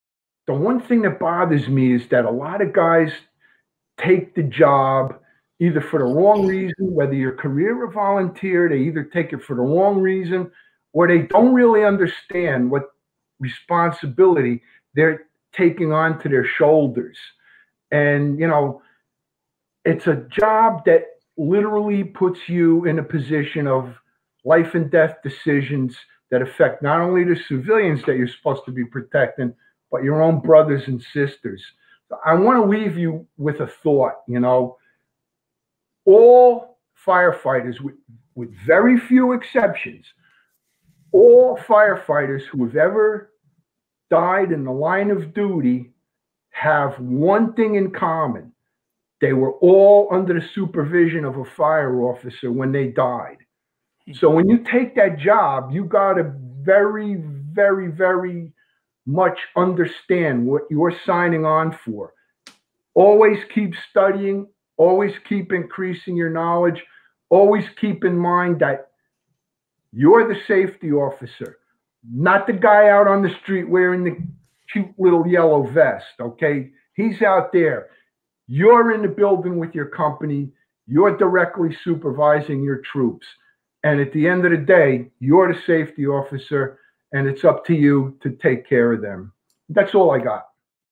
Company Officer Responsibilities Gettin' Salty Newark Chief of Rescue Operations Pete Partridge
Quoting Chief Partridge: 'With very few exceptions, all firefighters who have died in the Line of Duty have one thing in common: They were all under the  supervision of a Fire Officer when they died.' The Company Officer should understand the responsibility he is taking upon his shoulders. He is in a position to make life & death decisions that affect both citizen & firefighters lives. Every Officer should prepare himself & his men for the dangerous emergency scene, in particular by training, increasing his knowledge, and always keeping his head in the job. The Company Officer is his Company's Safety Officer. It's up him to keep the members under his command safe.